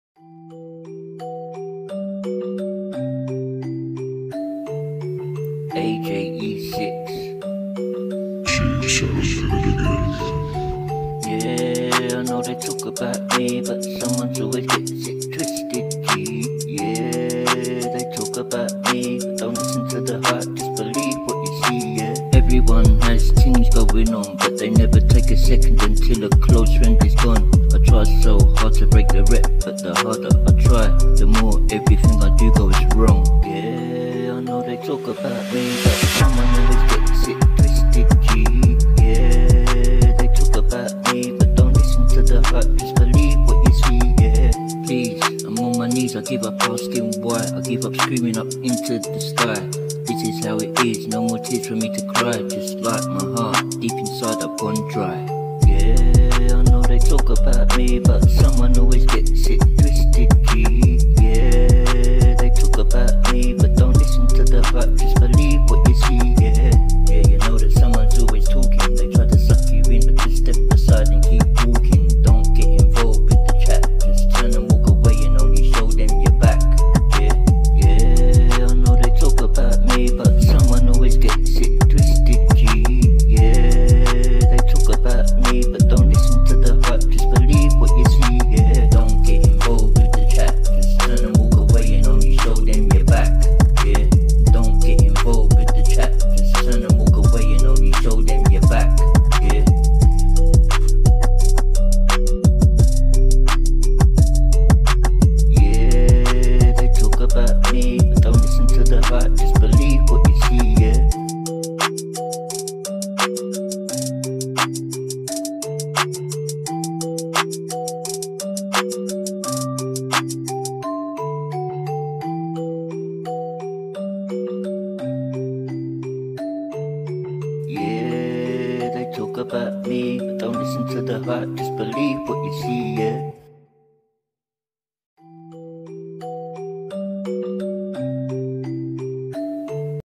UNBELIEVABLE, I made this on my mobile phone
been getting back into my music since losing my home, studio, health and brother in law, so not perfect but a small step in the right direction.